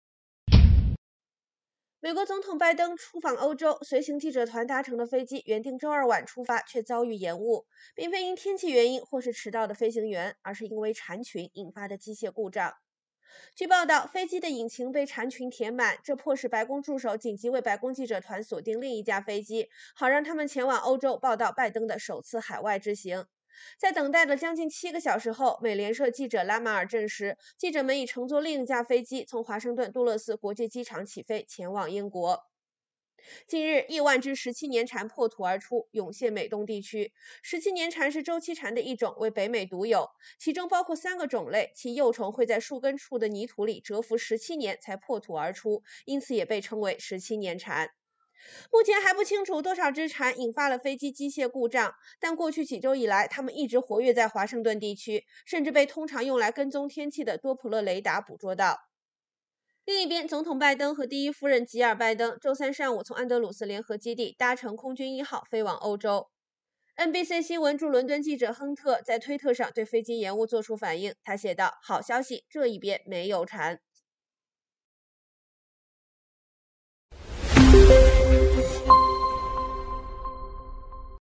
白宫记者团跟随拜登访英 飞机却因蝉延误近7小时
总统拜登出访欧洲，随行记者团搭乘的飞机原定周二晚出发，却遭遇延误，并非因天气原因或是迟到的飞行员，而是因为蝉群引发的机械故障。

据报道，飞机的引擎被蝉群填满，这迫使白宫助手紧急为白宫记者团锁定另一架飞机，好让他们前往欧洲报道拜登的首次海外之行。

在等待了六个多小时后，美联社记者拉马尔（Jonathan Lemire）证实，记者们已乘坐另一架飞机从华盛顿杜勒斯国际机场起飞前往英国。

近日，亿万只“17年蝉”破土而出，涌现美东地区。17年蝉是周期蝉的一种，为北美独有，其中包括三个种类，其幼虫会在树根处的泥土里蛰伏17年才破土而出，因此也被称为“17年蝉”。

目前还不清楚多少只蝉引发了飞机机械故障，但过去几周以来它们一直活跃在华盛顿地区，甚至被通常用来跟踪天气的多普勒雷达捕捉到。

总统拜登和第一夫人吉尔周三上午从安德鲁斯联合基地搭乘空军一号飞往欧洲。

NBC新闻驻伦敦记者亨特(Molly Hunter)在推特上对飞机延误做出反应，她写道：“好消息:这一边没有蝉!”